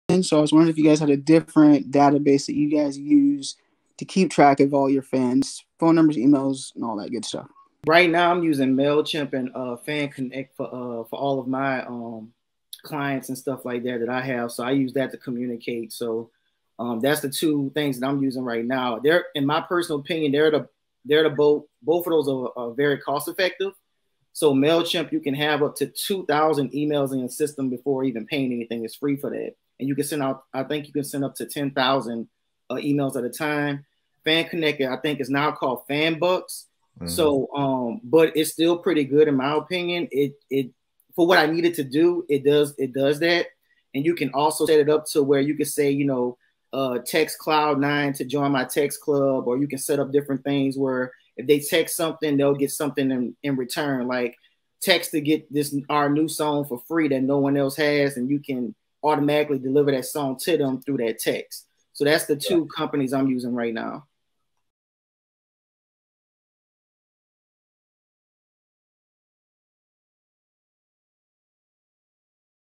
Top Marketing Databases For Storing Emails And Phone Numbers | KloudNineMusic
KloudNineMusic discusses the best platforms to use for collecting emails and phone numbers for music marketing.